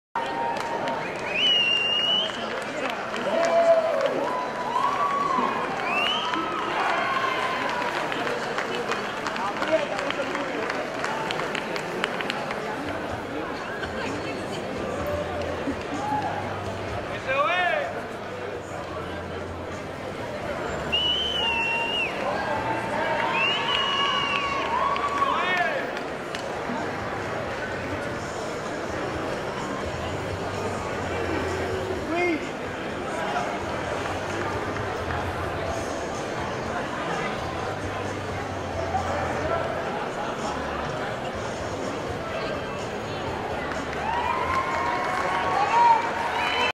NPC National Championships 2014 — Choi's Individuals / BB Open Heavyweight Class (21-22 Nov 2014)
CHOICE ATHLETICS™

• NPC National Championships (2014) — 16th Place, Men's Bodybuilding Open Heavyweight Class.
Official Total Score: ?.
Official Contest Weight: 221 lbs.
— 21-22 Nov 2014, Competitor Badge #179, Placing 16/37.